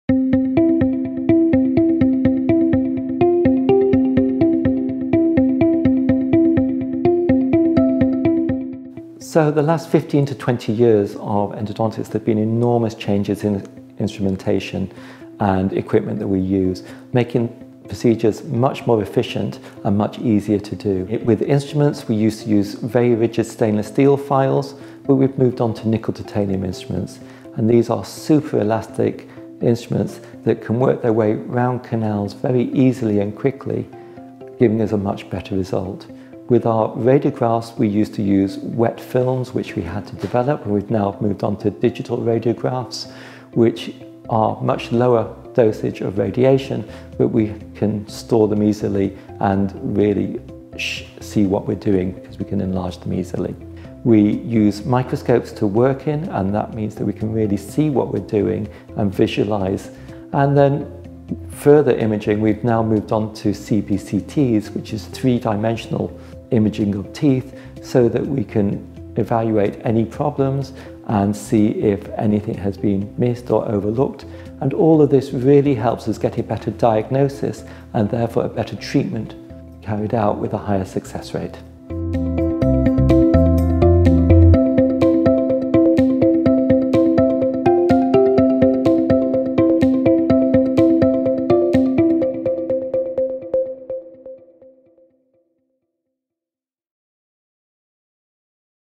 What are the cutting-edge technologies used by endodontists?
Endodontics is a specialist branch of dentistry that uses new instruments and technologies to carry out procedures such as root canals. Learn more with expert endodontist Dr Michael Sultan